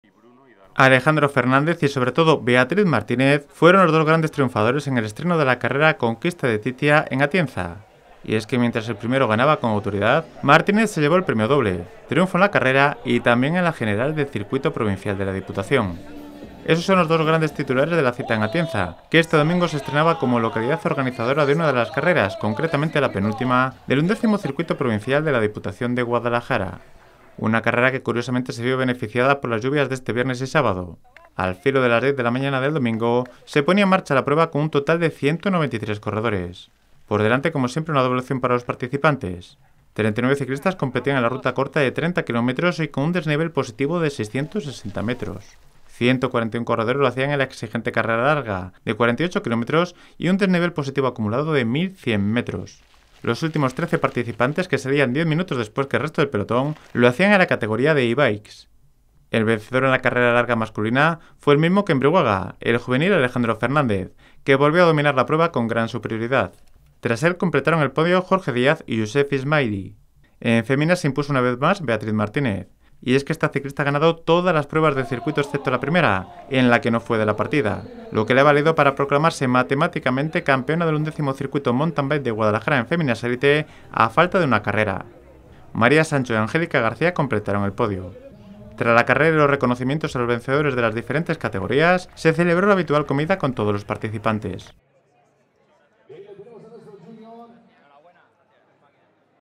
Beatriz Martínez, victoria doble en Atienza
Alejandro Fernández y sobre todo, Beatriz Martínez fueron los grandes triunfadores en el estreno de la Carrera Conquista de THYTIA en Atienza, ya que Martínez conquistó también el Circuito Provincial.